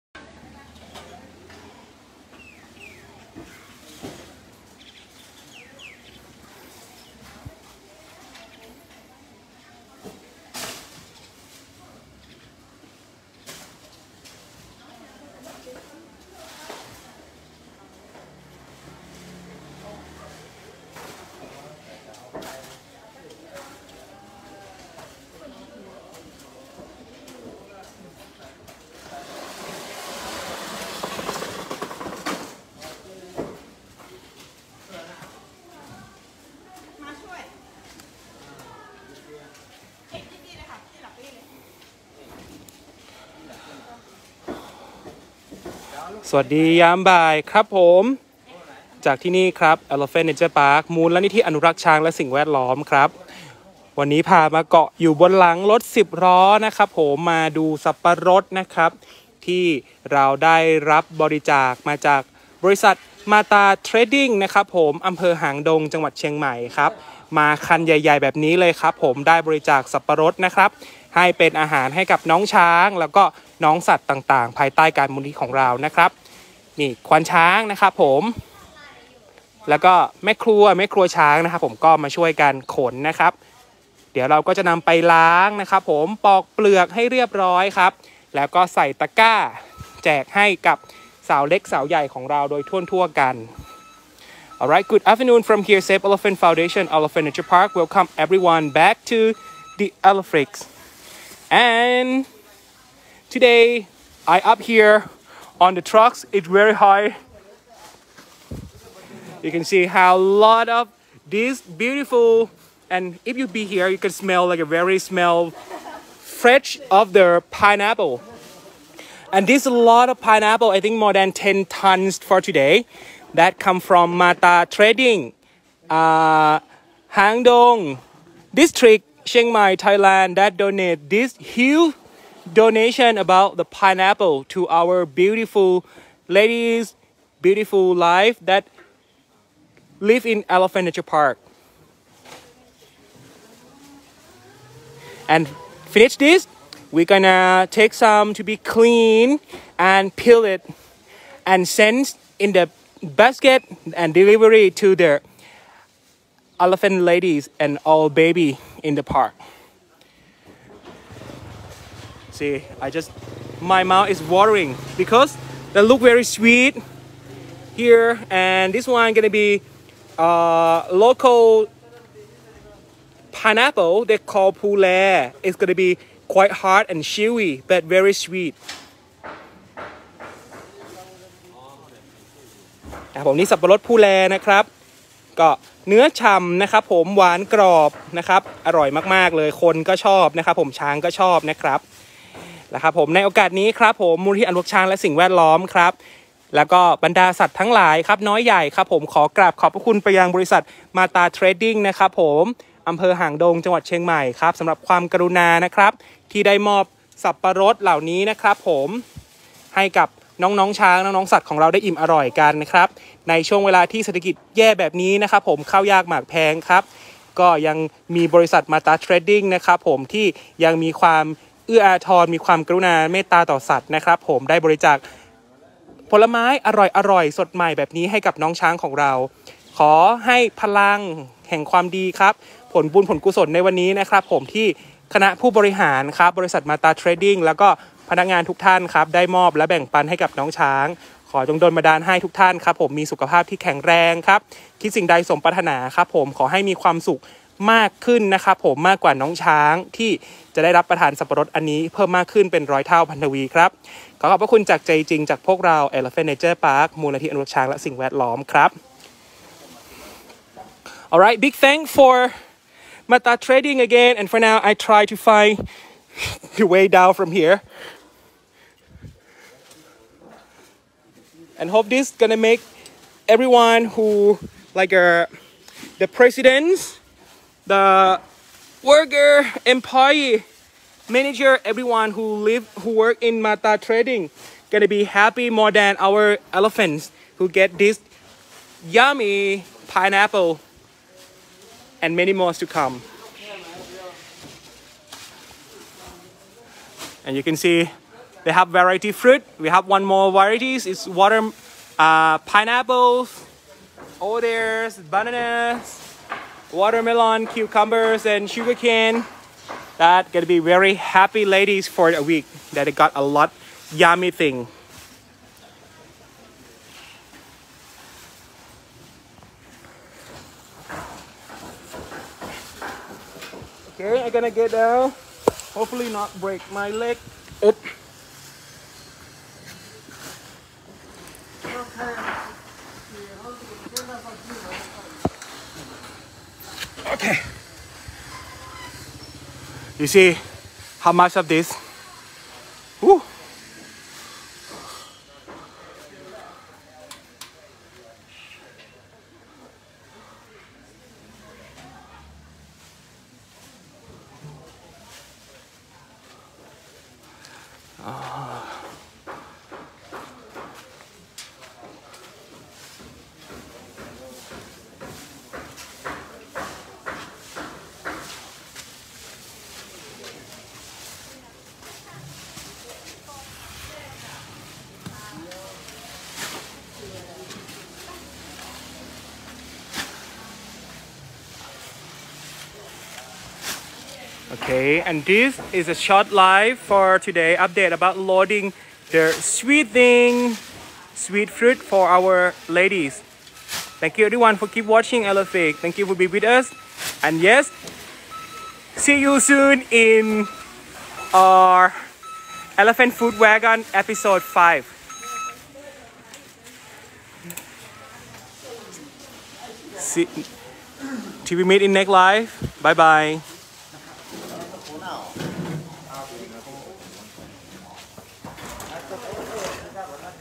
Big thank to Matatrading for this big donation 🙏🏻❤️🐘 - EleFlix
This video was released on 18/6/2022   on Eleflix Facebook Live.
Hello everyone welcome to meet with Eleflix Chanel.
กราบขอบพระคุณ บริษัท มาตาเทรดดิ้ง จำกัด อ.หางดง จ.เชียงใหม่ สำหรับ สับปะรดเป็นอาหารให้กับน้องช้างและบรรดาสัตว์ มูลนิธิอนุรักษ์ช้างและสิ่งแวดล้อม